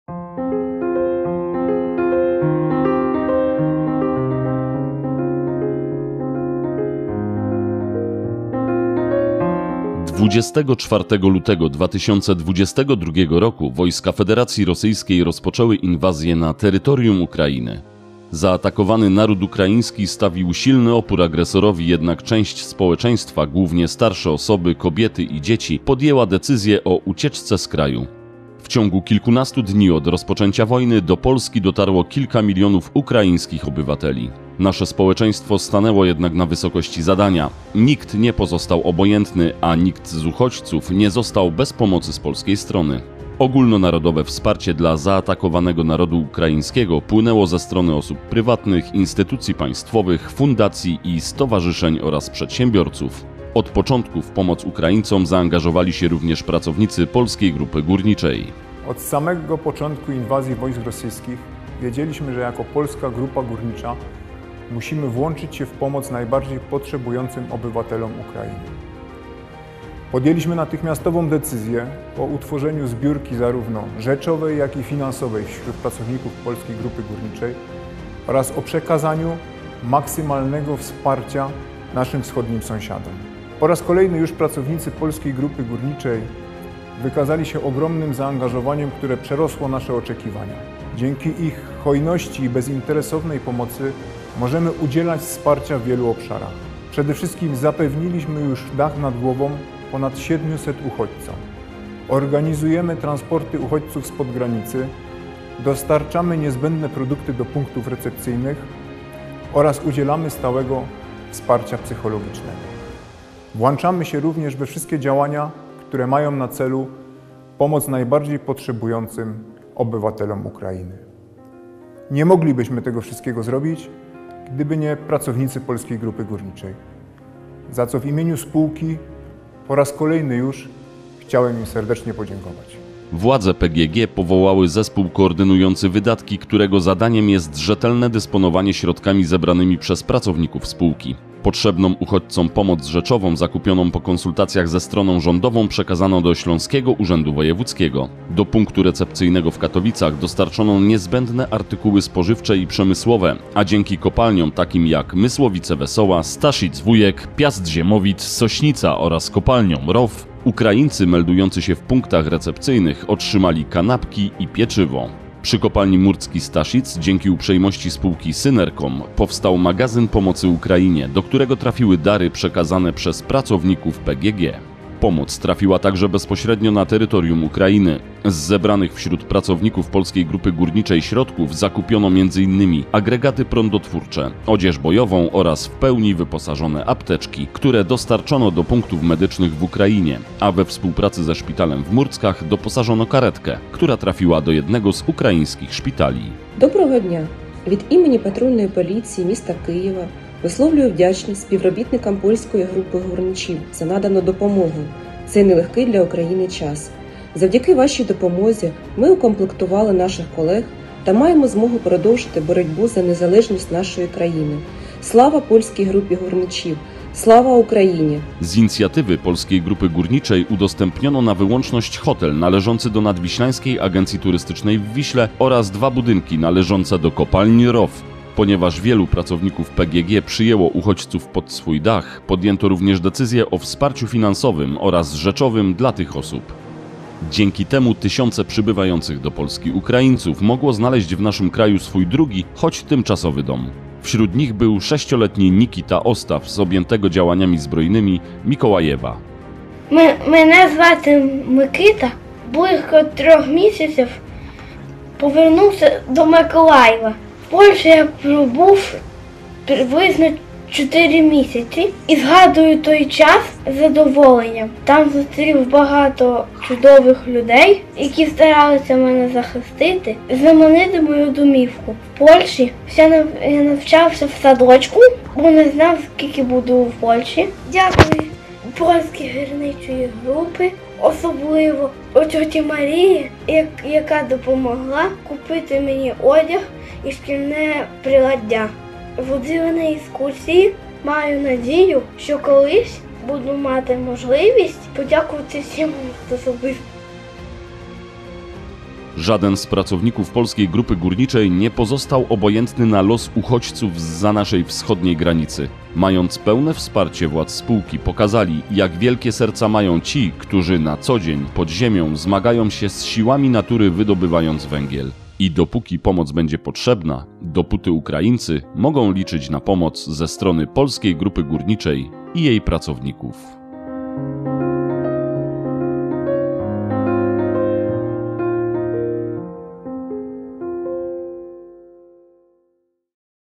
Pracownicy PGG S.A. solidarni z Ukrainą 2022
Od pierwszych chwil po wybuchu wojny na Ukrainie, kiedy miliony ukraińskich kobiet i dzieci ruszyły na zachód w poszukiwaniu bezpieczeństwa, z pomocą uchodźcom wojennym z Ukrainy pospieszyli m.in. pracownicy Polskiej Grupy Górniczej S.A. Natychmiast podjęto decyzję o utworzeniu zbiórki rzeczowej i finansowej wśród załogi PGG S.A.

Zaangażowanie górników przerosło oczekiwania. Tylko w pierwszym miesiącu wojny, przez dwa tygodnie marca, pracownicy PGG S.A. wpłacili niemal 370 tys. zł z przeznaczeniem na pomoc dla ukraińskich uchodźców wojennych. Dzięki bezinteresownej pomocy pracowników i ich hojności, dach nad głową udało się zapewnić ponad 700 uchodźcom, organizowano też transporty spod granicy na Górny Śląsk, do gmin, w których mieszkają zatrudnieni w PGG S.A.

Do punktów recepcyjnych dostarczane są produkty żywnościowe i pierwszej potrzeby, kupione przez pracowników spółki. Zorganizowano też stałe wsparcie psychologów.

Pieniądze z funduszu pomocy pracowników PGG S.A. dla uchodźców wydawane są pod nadzorem Zespołu Koordynującego Wydatki, którego zadaniem jest rzetelne dysponowanie zebranymi przez pracowników środkami. Pomoc rzeczową kupuje się po konsultacjach ze stroną rządową, aby najlepiej dopasować ją do bieżących potrzeb. Do punktu dystrybucji Śląskiego Urzędu Wojewódzkiego w Katowicach trafiają artykuły spożywcze i przemysłowe. Dzięki pracownikom kopalń Mysłowice-Wesoła, Staszic-Wujek, Piast-Ziemowit, Sośnica i ROW, ludzie w punktach dla uchodźców, otrzymywali m.in. kanapki i pieczywo. Przy kopalni Murcki-Staszic we współpracy ze spółką Synercom utworzono magazyn pomocy Ukrainie, dokąd trafiają dary przekazane przez pracowników PGG S.A.

Pomoc dotarła też bezpośrednio na terytorium Ukrainy. Za zebrane wśród pracowników pieniądze kupiono m.in. agregaty prądotwórcze, odzież polową i kompletnie wyposażone apteczki. We współpracy ze szpitalem w Murckach doposażono karetkę pogotowia ratunkowego, która służy w jednym z ukraińskich szpitali. Tysiące kobiet i dzieci znalazło schronienie w udostępnionym im hotelu NAT w Wiśle i dwóch budynkach należących do kopalń rybnickich ROW, lecz przede wszystkim w domach pracowników PGG S.A., którzy przyjęli rodziny uciekające przed okropnościami wojny.

Dzięki pośrednictwu pracowników PGG S.A. w programie Akademia PGG ponad 2 tys. uchodźców z Ukrainy skorzystało z możliwości darmowej nauki języka polskiego na internetowej platformie eTutor, która działa od dawna na portalu pracownika PGG S.A. (oferując kursy angielskiego, niemieckiego i hiszpańskiego dla pracowników i ich rodzin).

Niektóre z rodzin uchodźców po kilku miesiącach wróciły szczęśliwie do wyzwolonych już miast, jak np. Mikołajów i ślą serdeczne podziękowania za okazaną im w Polsce pomoc. Nikt z pracowników PGG S.A. nie pozostaje obojętny na los ludzi pokrzywdzonych napaścią Rosji na Ukrainę, akcja pomocy pracowników PGG S.A. dla uchodźców wojennych ciągle trwa.

Okazją do podsumowania dotychczasowego etapu akcji pomocy pracowników PGG S.A. "Solidarni z Ukrainą" była uroczysta Akademia Barbórkowa Polskiej Grupy Górniczej z koncertem, na którym 3 grudnia 2022 r. w Katowicach przed pracownikami i gośćmi zagrali młodzi ukraińscy muzycy Młodzieżowej Akademickiej Orkiestry Symfonicznej Ukrainy "Słobożański" z Charkowa, która po inwazji Rosjan na Ukrainę straciła możliwość koncertowania w zniszczonym mieście. Nowy artystyczny dom młodzi muzycy tymczasowo znaleźli w Polsce przy wparciu Ministerstwa Kultury i Dziedzictwa Narodowego oraz Instytucji Kultury Katowice - Miasto Ogrodów.